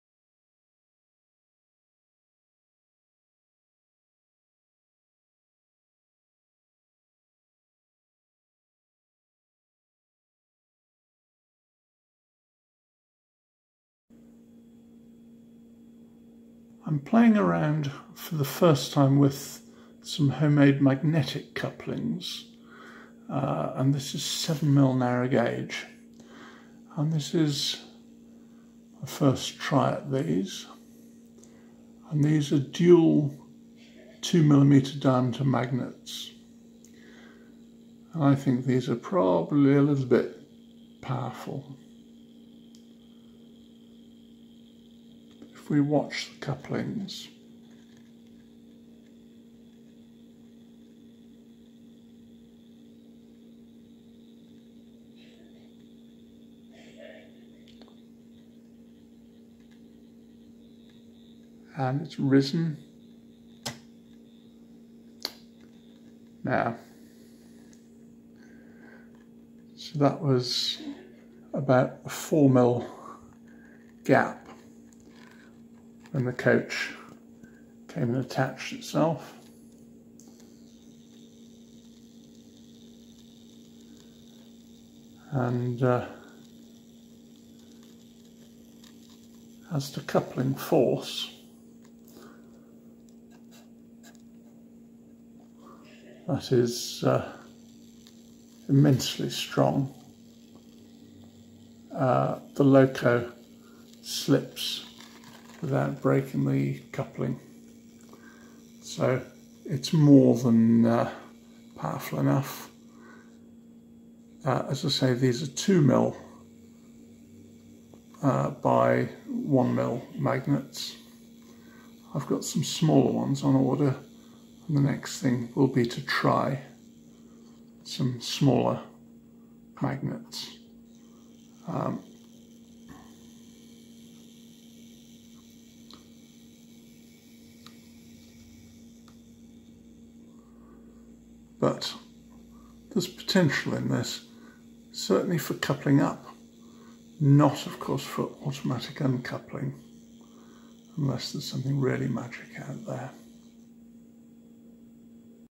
Magnetic Coupling experimental for 7mm narrow gauge
I'm aware of the various coupling systems that are available out there. To date I have used choppers and then pin-and-link. Choppers are not good on tight radii, and pin and link have no flexibility regarding coupling and uncoupling. I'm therefore having a play.